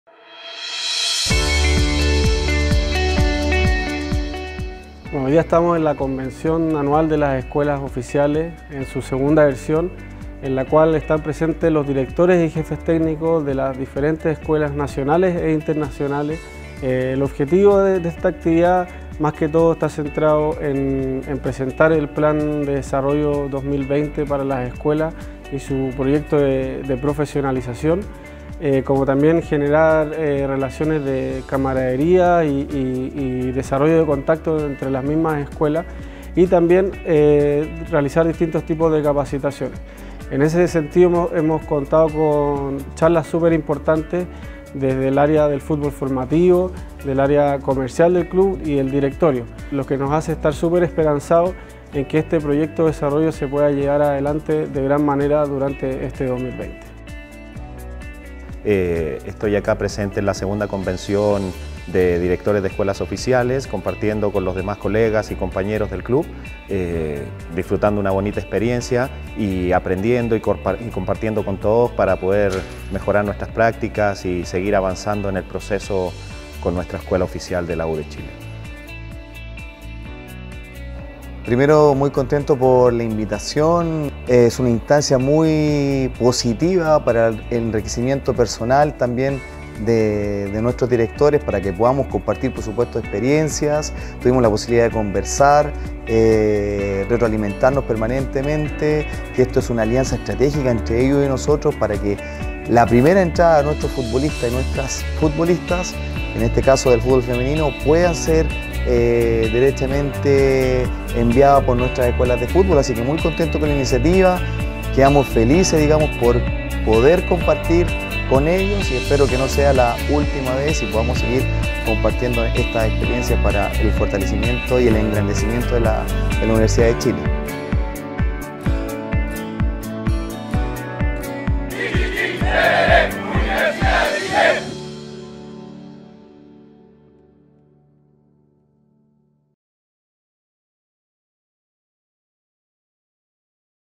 Escuelas Oficiales: Convención Anual de Directores 2020
El pasado sábado, el Centro Deportivo Azul recibió una nueva Convención de Escuelas Oficiales con miras al año 2020. Con la presencia de 40 sedes, incluyendo a representantes de las asentadas en México y Estados Unidos, se llevó a cabo esta importante reunión, donde los más de 60 participantes pudieron conocer el plan estratégico para el presente año, y donde además, pudieron capacitarse para contribuir en el cumplimiento del Rol de las distintas escuelas con el Club. Revisa las mejores imágenes de la jornada. ⠀